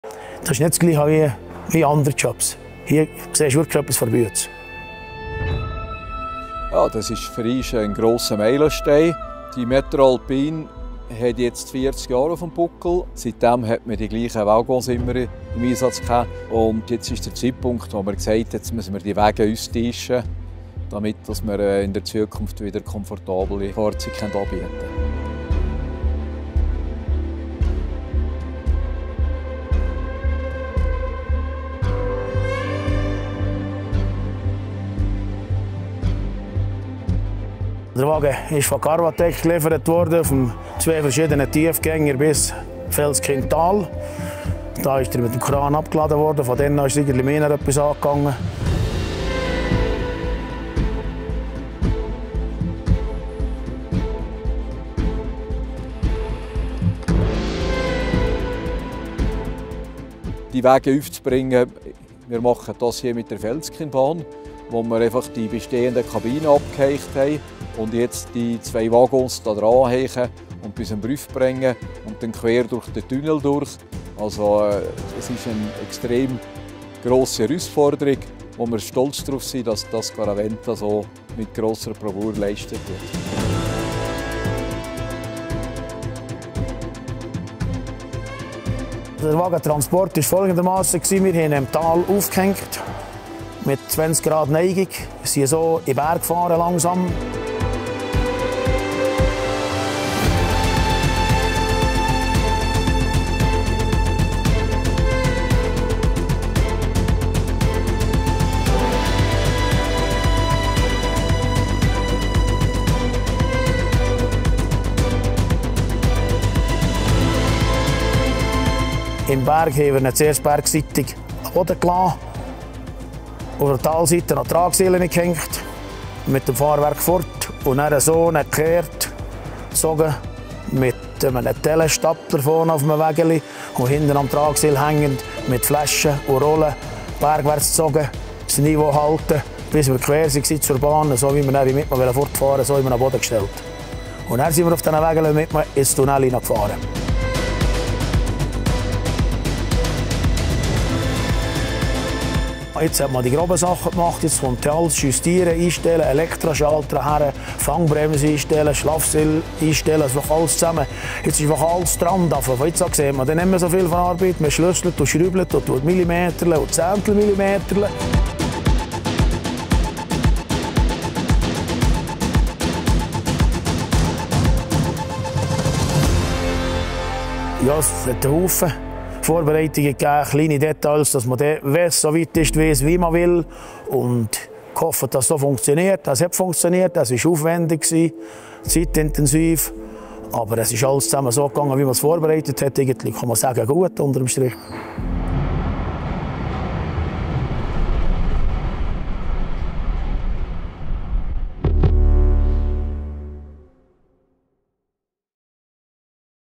Garaventa 95-FUL Felskinn-Mittelallalin "Metro Alpin" Saas Fee, Schweiz - Umbau (2024)
Seit der Eröffnung im Jahr 1984 bietet die höchste unterirdische Standseilbahn der Welt in Saas Fee ein einzigartiges Transporterlebnis für Einheimische und Besucher. Diese Standseilbahn verbindet die Station Felskinn mit dem Mittelallalin auf rund 3500 m.ü.M.

Nach 40 Jahren wurden die alten Fahrzeuge nun durch moderne Modelle von Carvatech ersetzt, um den höchsten Standards an Sicherheit und Komfort gerecht zu werden. Dieses Video gibt dir einen Einblick in den komplexen Prozess des Fahrzeugwechsels: Von der Anlieferung auf Spezialanhängern, über das Entladen, die präzise Montage, bis hin zu den Feinjustierungen im Tunnel an ihrem Bestimmungsort.